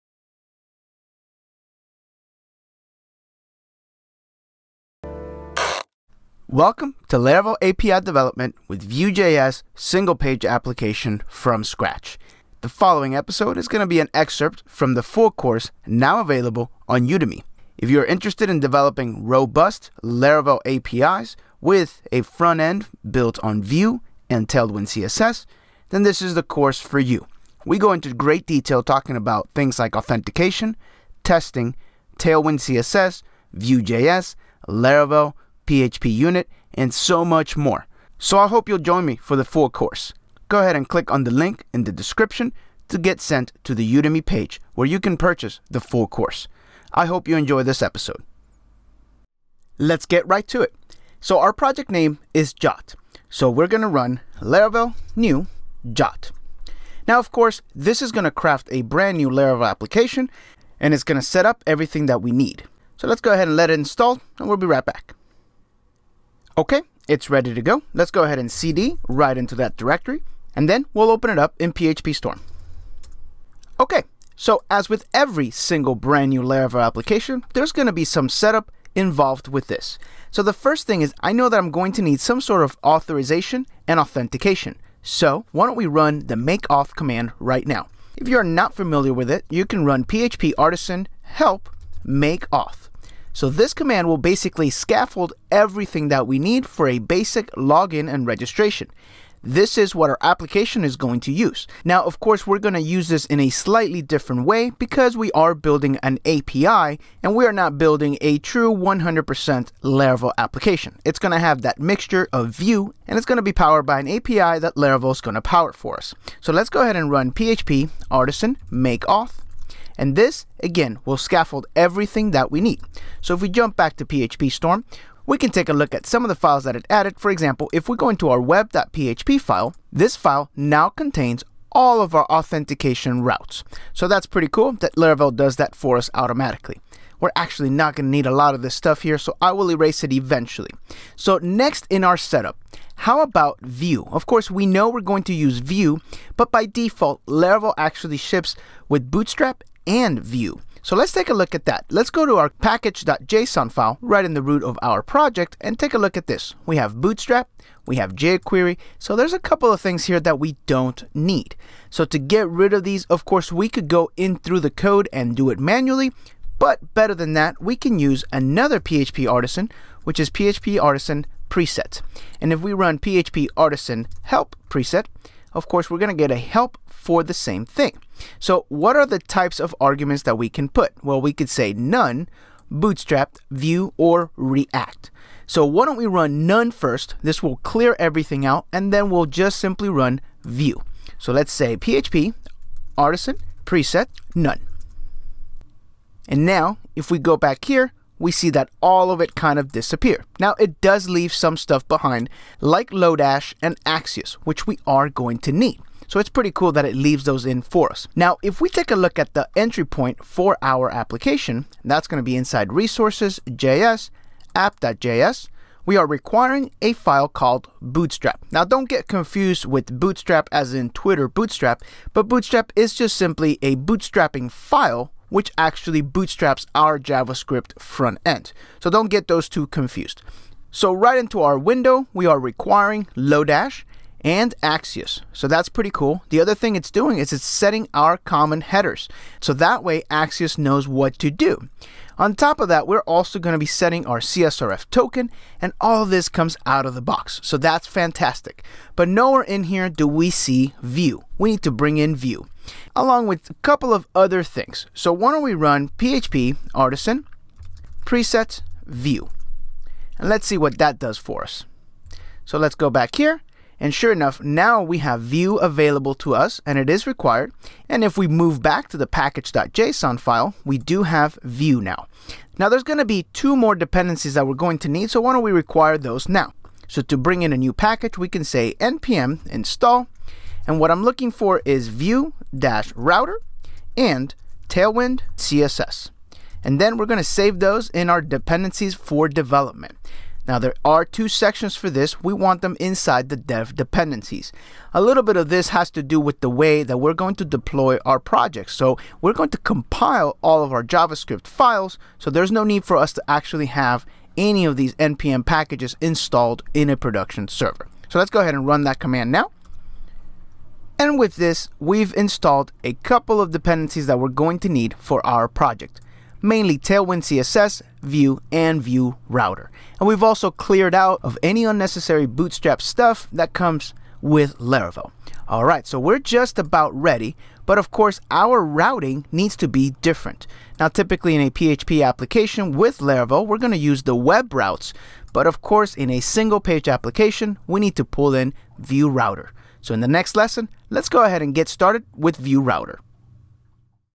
Laravel API Development & Vue JS SPA - e01- Project Setup
To get started, let's install a fresh installation of Laravel and get working on setting up our project.

In this episode, we cover:

Scaffolding of new Laravel project

Scaffolding of authentication

Strip all of the default front-end

Install Vue, Vue Router & Tailwindcss using NPM

Learn how to develop a robust API with Laravel and a Single-Page Application in Vue JS from Scratch

- RESTful API Development with Laravel
- Vue JS Single Page Application Methodology
- Front-End Design Using Tailwind CSS
- Implementing Search Functionality Using Laravel Scout
- Build a Complete SPA from Scratch